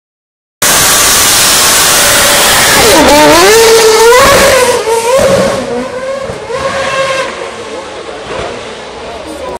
Bavaria City Racing 2012 - Button take off
Button's MP4-26 leaving O Connell's bridge in a trail of smoke and noise!